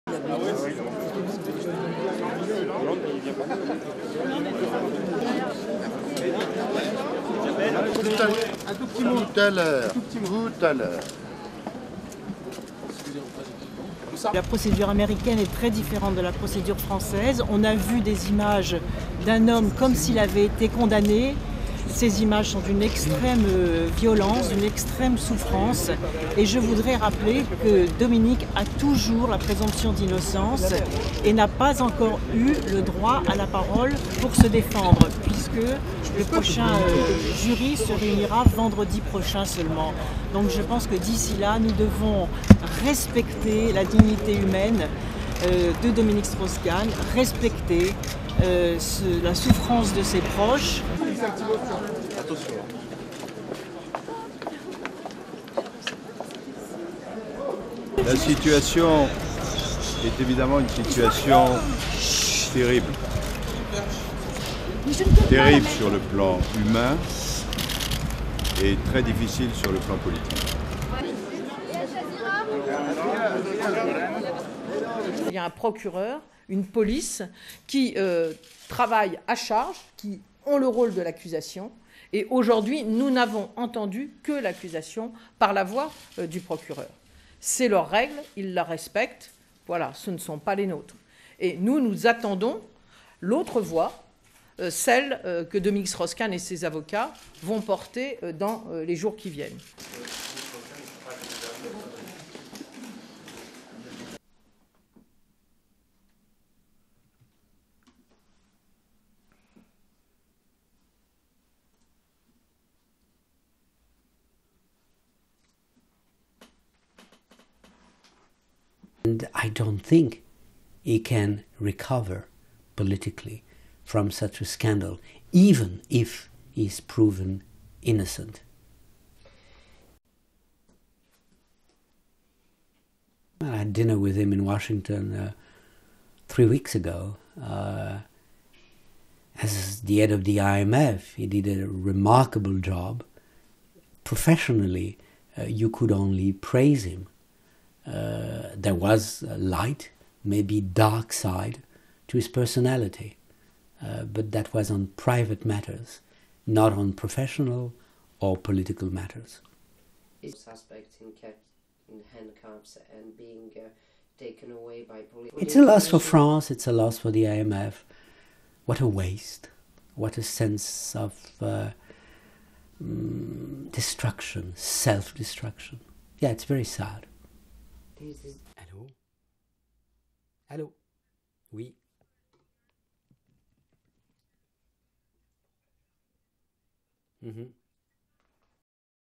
Socialist leaders, analyst, react to detention of IMF chief
1. Wide pan of crowd of journalists inside courtyard of Socialist Party headquarters
2. Arrival of Paris Mayor, Bertrand Delanoe
3. SOUNDBITE: (French) Segolene Royal, French former Socialist Party presidential candidate:
"American judicial proceedings are very different from those in France. We have seen the images of a man looking as if he has already been convicted. These images are extremely violent and painful and I would like to remind everyone that Dominique is still presumed innocent. He has yet to have the right to speak and defend himself since the next jury session will be on Friday. So I think that until then we should respect Dominique's human dignity, respect the suffering of his loved ones."
4. Arrival of French parliamentarian, Pierre Moscovici
5. SOUNDBITE: (French) Laurent Fabius, Former French Prime Minister:
"This is obviously a terrible situation - terrible on a human level and very difficult on a political level."
6. Crowd of journalists outside Socialist Party headquarters
7. SOUNDBITE: (French) Martine Aubry, leader of French Socialist Party:
"There is a prosecutor and police detectives who are working together to build a case, and today we have only heard the accusations from the voice of the prosecutor. That's their rules. We respect them. They aren't ours and we are waiting for the other voice, that of Dominique Strauss-Kahn and his lawyers, and that will happen in the coming days."
8. Aubry leaving podium
9. Wide of political analyst, Dominique Moisi
10. Tilt up of Moisi at computer
11. SOUNDBITE (English) Dominique Moisi, Political Analyst:
"I don't think he (Strauss-Kahn) can recover politically from such a scandal, even if he is proven innocent."
12. Cutaway of Moisi
13. SOUNDBITE (English) Dominique Moisi, Political Analyst:
"I had dinner with him (Strauss-Kahn) in Washington three weeks ago. As the head of the IMF he did a remarkable job. Professionally you could only praise him. There was a light and maybe a dark side to his personality but that was on private matters, not on professional or political matters."
14. Close up of Moisi
15. SOUNDBITE (English) Dominique Moisi, Political Analyst:
"It's a loss for France, it's a loss for the IMF. What a waste, what a sense of destruction, self-destruction. Yes, it's very sad."
16. Moisi on phone, pan to cat on chair
STORYLINE
Members of France's Socialist party were in shock on Tuesday, while President Nicolas Sarkozy reportedly urged his fellow conservatives to contain their glee, after U.S. sexual assault charges sidelined his opposition's leading likely contender in next year's presidential race.
Although leading French politicians are loath to say so out loud, Sarkozy may gain political leverage from the detention on rape charges of IMF chief and prominent Socialist Dominique Strauss-Kahn, known in France as DSK.
Strauss-Kahn, who has been charged with attempting to rape a hotel maid in New York, was jailed without bail after a publicly broadcast arraignment on Monday that shocked many in France.
It's illegal to broadcast images of suspects in handcuffs in France out of respect for the principle of the presumption of innocence until proven otherwise.
"We have seen the images of a man looking as if he has already been convicted. These images are extremely violent and painful and I would like to remind everyone that Dominique is still presumed innocent," said Segolene Royal, who, as the Socialist Party presidential candidate in 2007, was beaten by Sarkozy.
Royal was speaking after a meeting of the Socialist Party leadership on Tuesday.